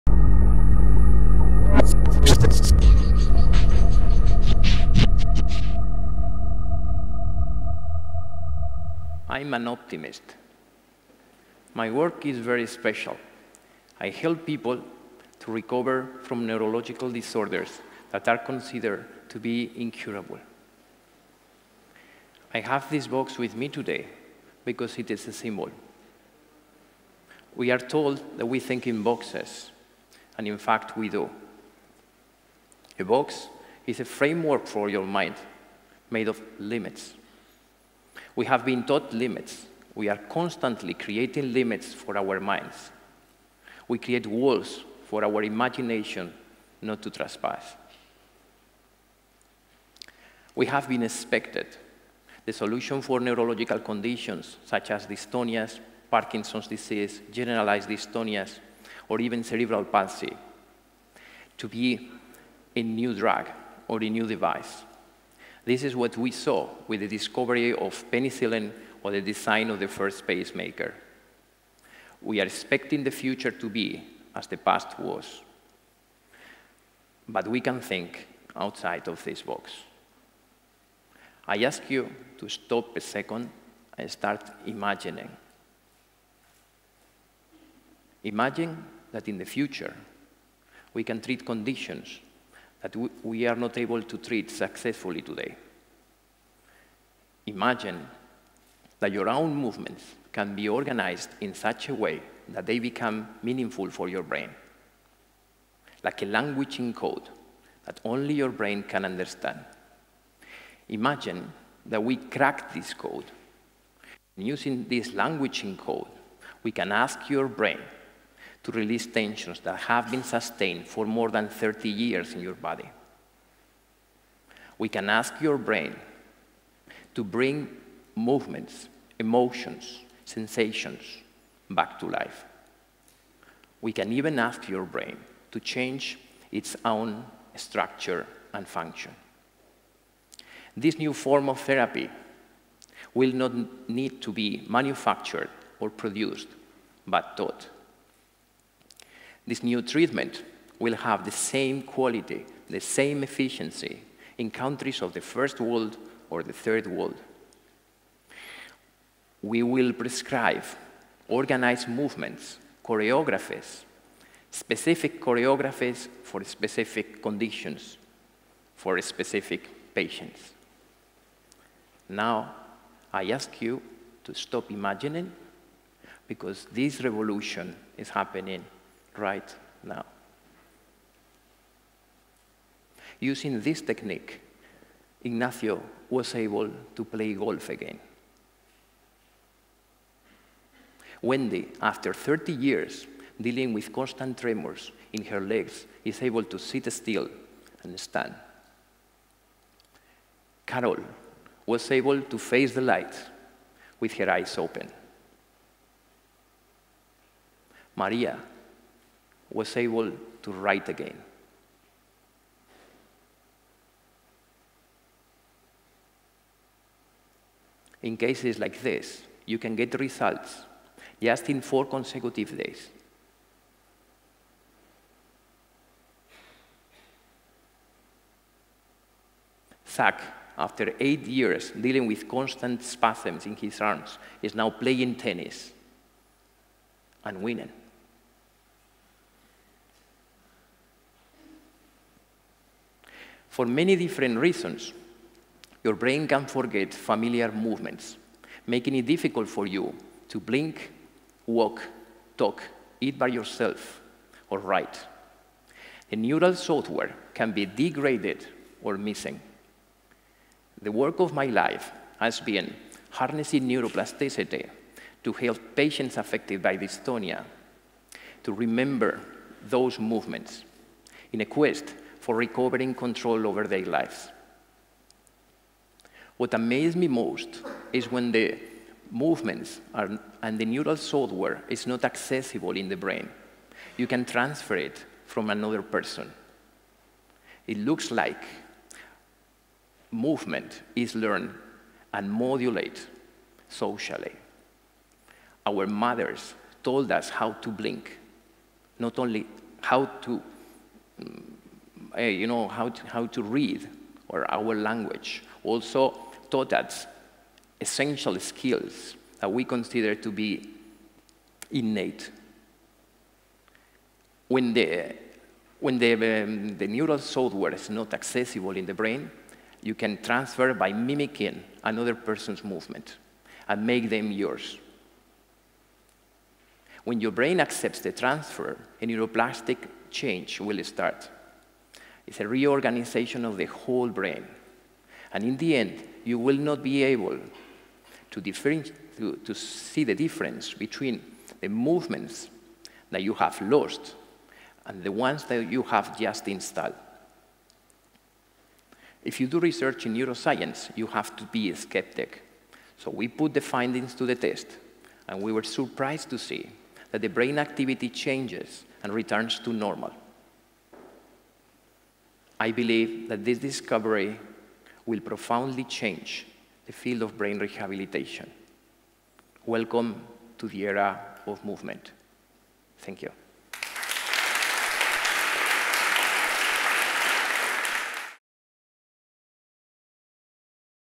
How your movements can heal your brain | Joaquin Farias | TEDxNapoli
Can certain neurological conditions make us forget how to move?  In this talk Dr. Farias explains how is possible to awaken silent  neural pathways in the brain, bringing lost movements, emotions and memories back to life.

Joaquin Farias is the director of the Neuroplastic Training Institute Toronto and researcher at the University of Toronto. Since 1996, He has been working on providing effective alternative and complementary care for patients affected by Dystonia and Parkinsons' Disease by pioneering new theories on neuroplasticity induced by movement therapy.